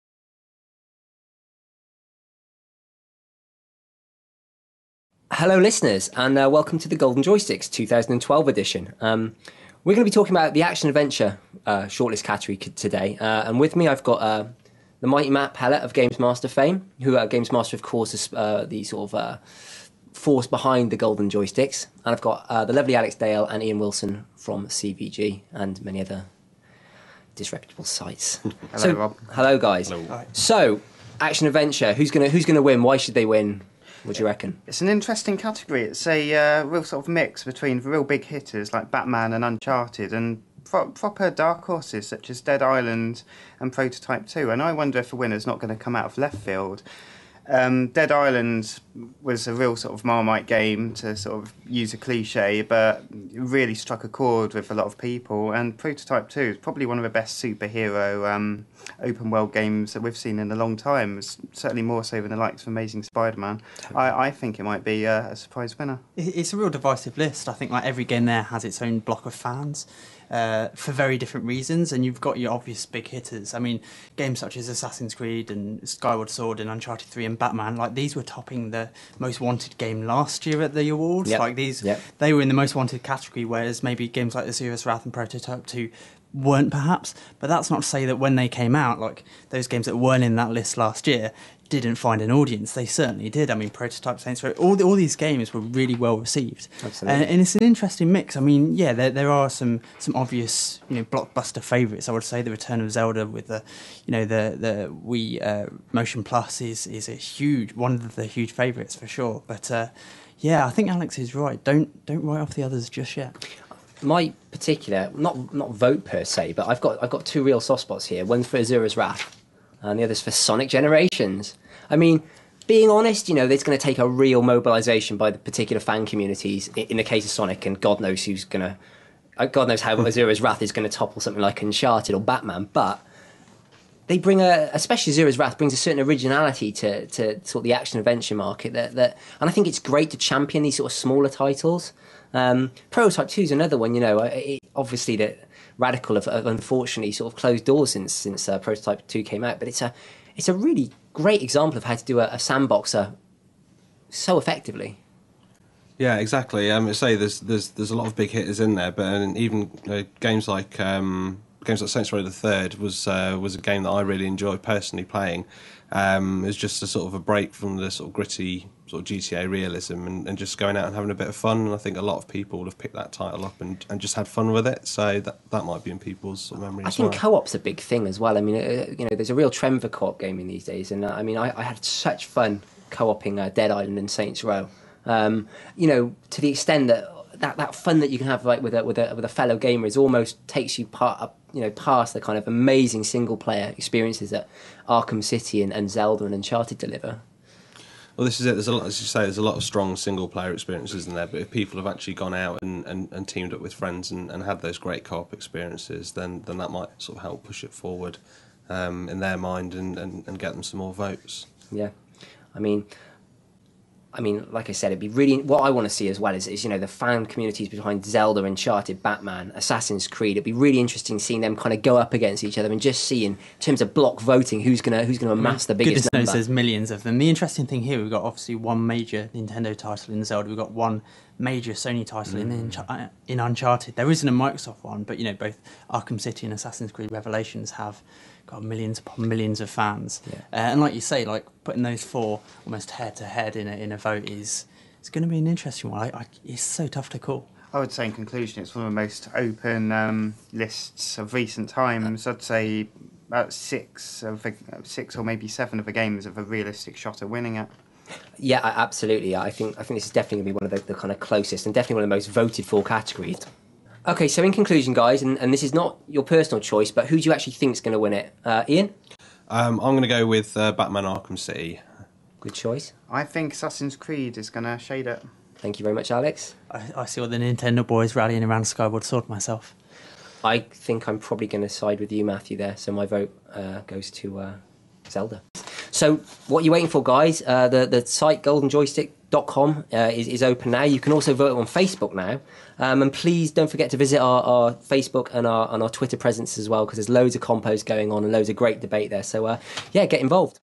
Golden Joystick Awards 2012: Best Action Adventure nominees
Golden Joystick Awards: Best Action Adventure nominees. Check out the nominees up for best action adventure at the 2012 Golden Joystick awards. Did your favourite make the shortlist?